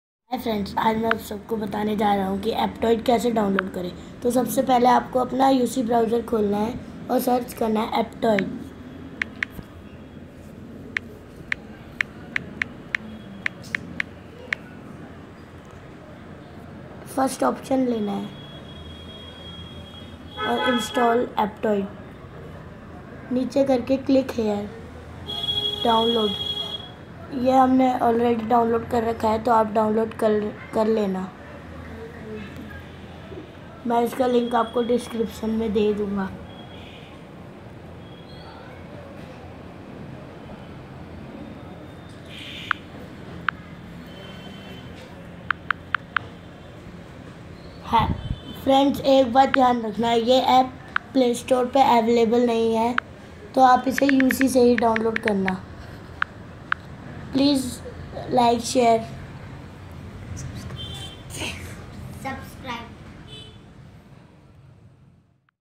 how to download aptoide in android / hindi by sam tech
aptoide download link

हेलो दोसतो इस विडियो मे मेने आपको कि aptoide को केसे download kare  .  aptoide playstore मे नहि हे  तो अाप इसे वेसे हि download करे जेसे मेने बाताया
by sam tech
Samtech